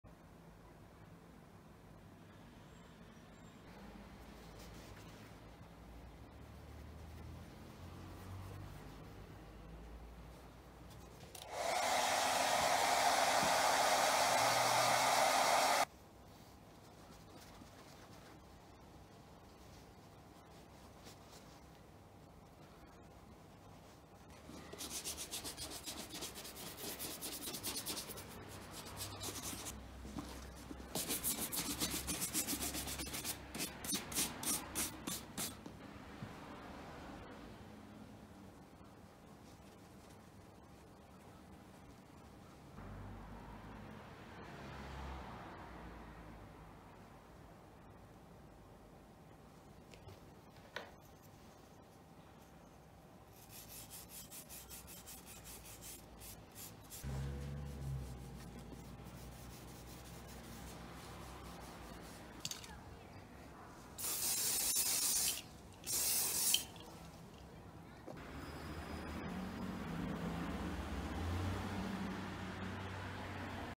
【靴磨き】お客様のウェスタンブーツを磨きます【shoeshine】
色落ちが目立つウェスタンブーツを磨きます。
無色のクリームから茶系のクリーム、仕上げにアニリンカーフクリームを使ってます。